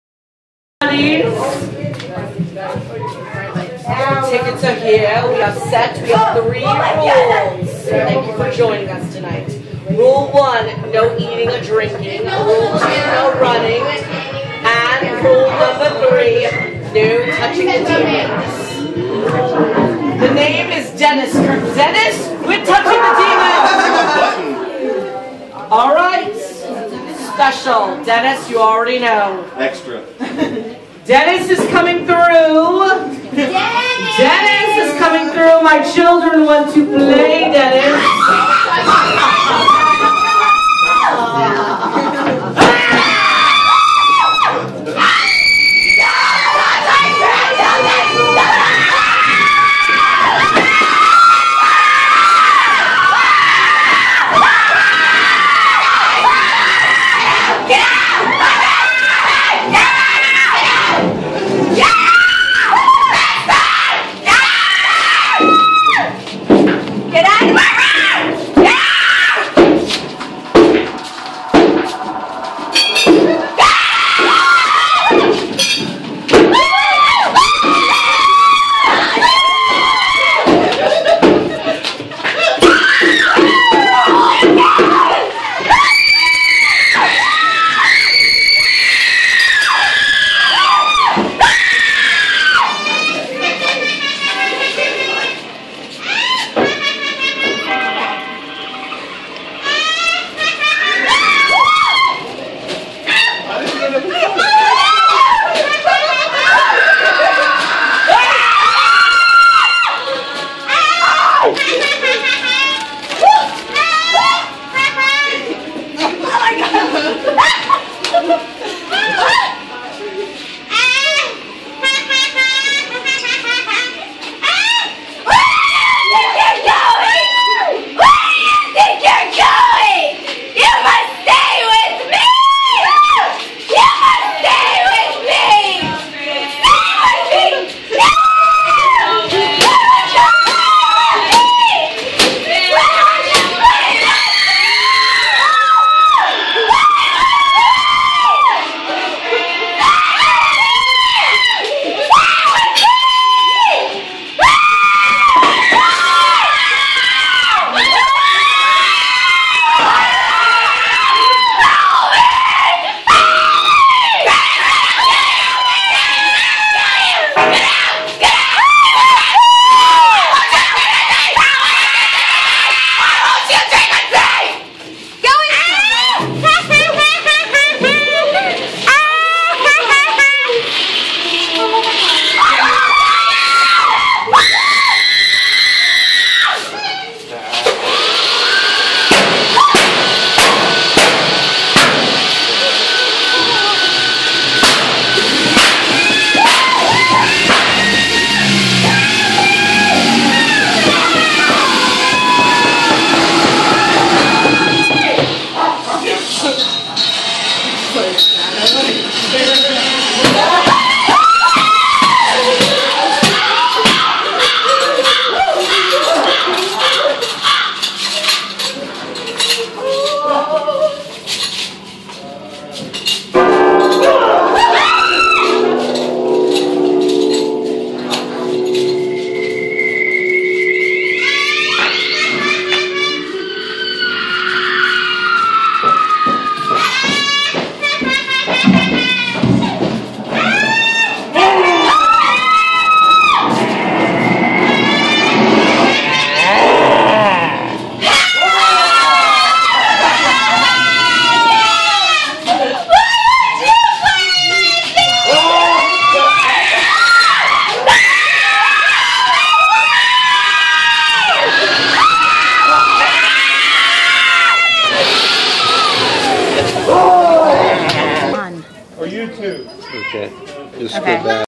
Margate Chamber 13
Haunted house, 13 rooms of terror. Sunday 20th, 25-27, and 31st.